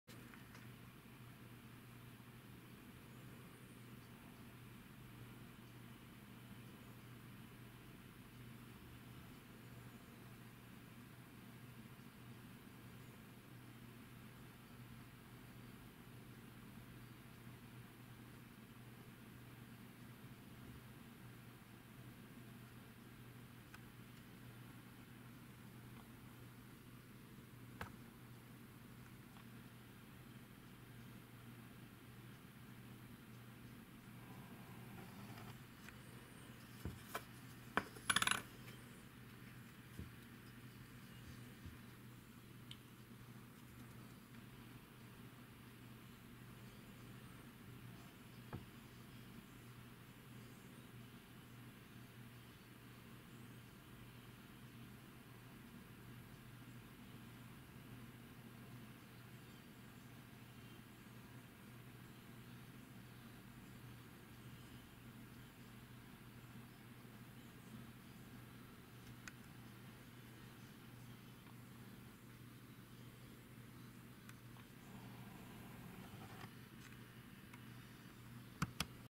3 копейки 1978 года
Обзор и увеличение монеты 3 копейки 1978 года выпуска. СССР. Рекомендовано к просмотру на большом экране ПК или на телевизоре